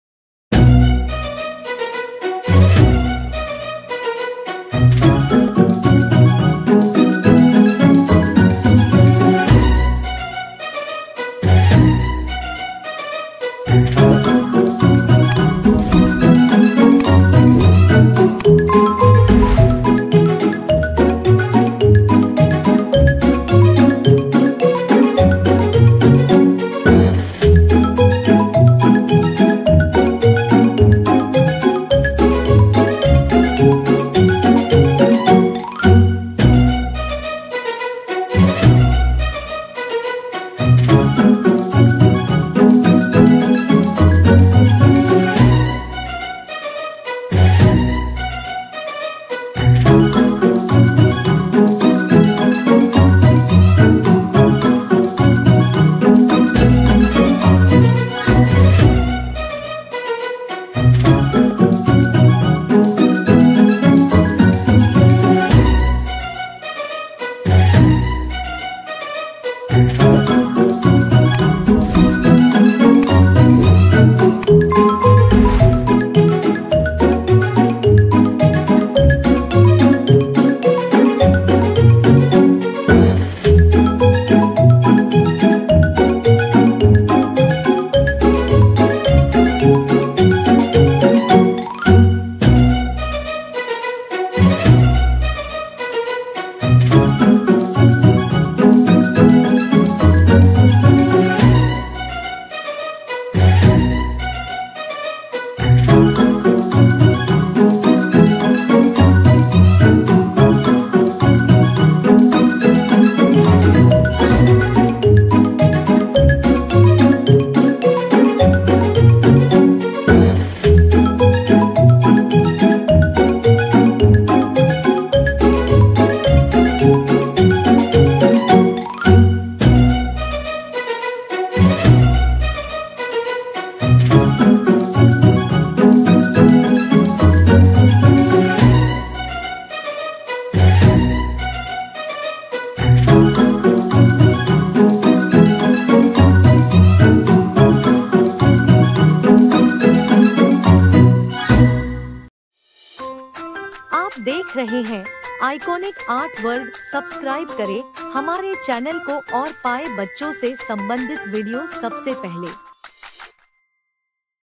How to draw Genie step by step for kids and toddlers
In this video, we learn to draw Spider Man, Genie, Princess Jasmine and Wonder Women step by step for kids and toddlers.